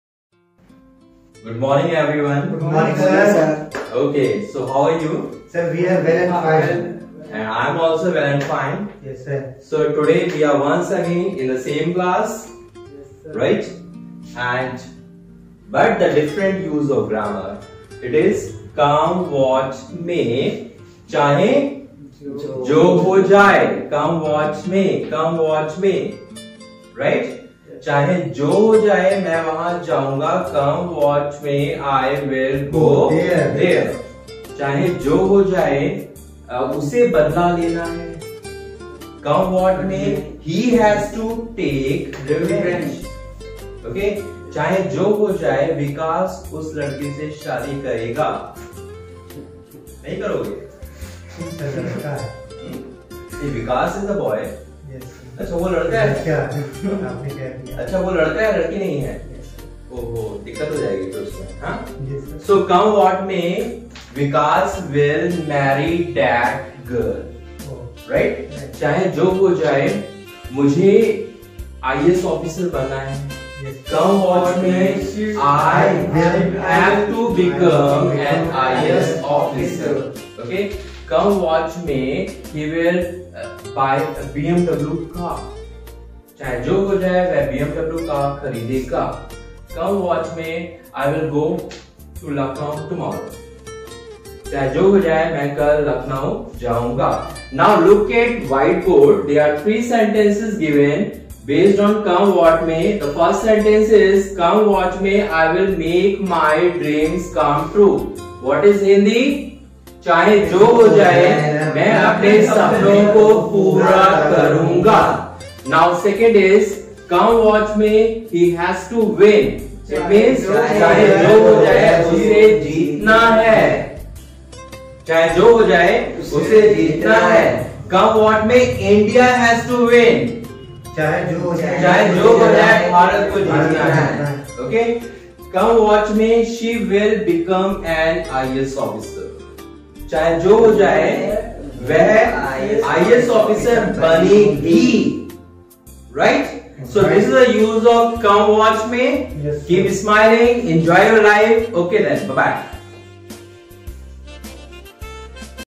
Use of come what may
This video is based on come what may,Many sentences were made on it,It is really very helpful for English learners.